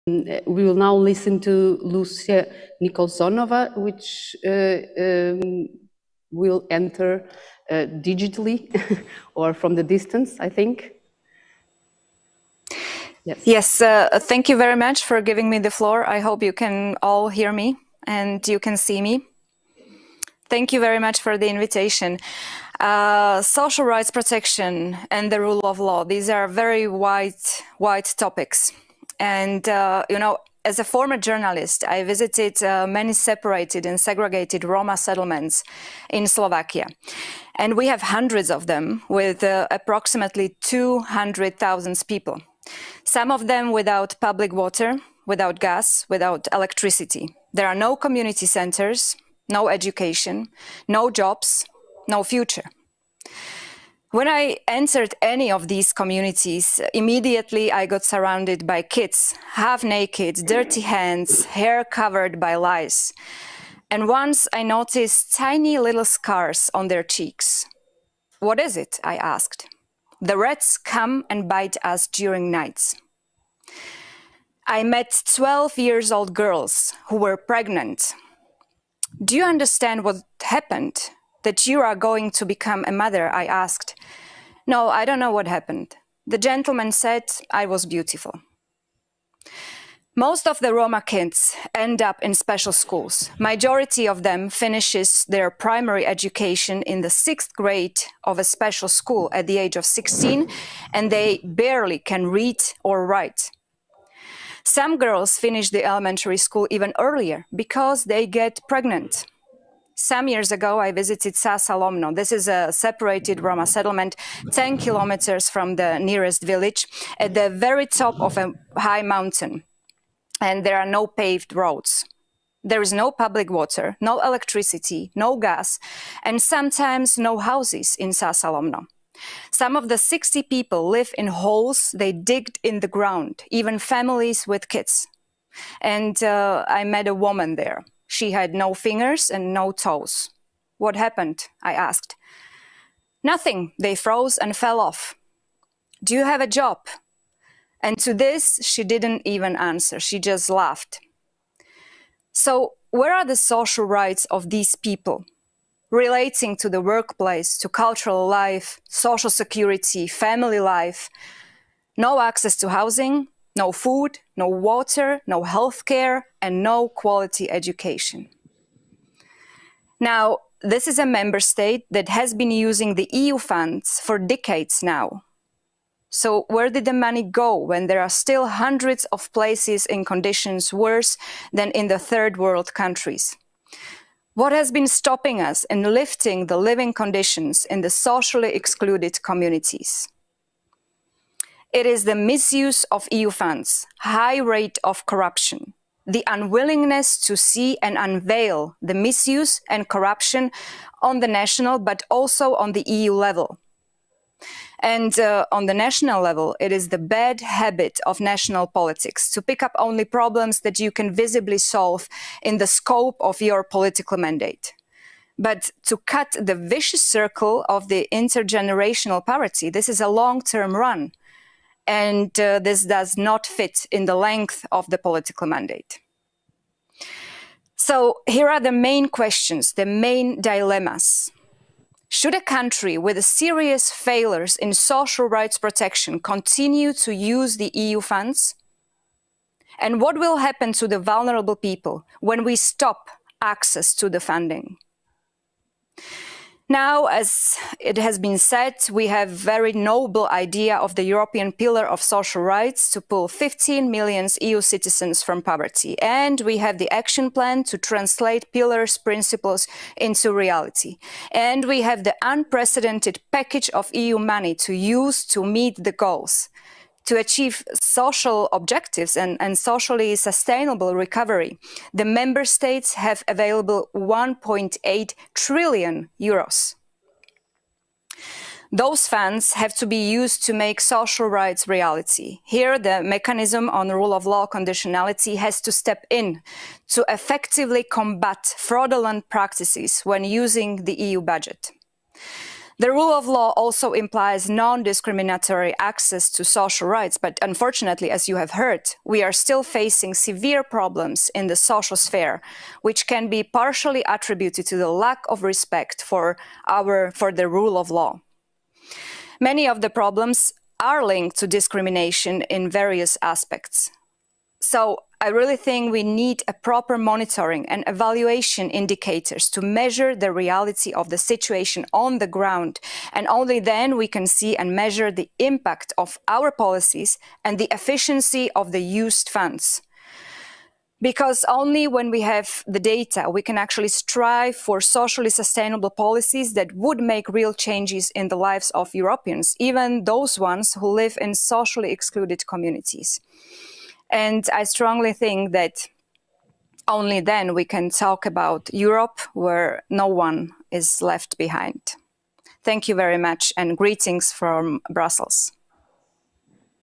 Deti bez vody a elektriny. Realita aj na Slovensku.
Vystúpenie na konferencii RULE OF LOW.
Príbehy majú konkrétne mená ľudí a ich osudy sú aj našimi osudmi.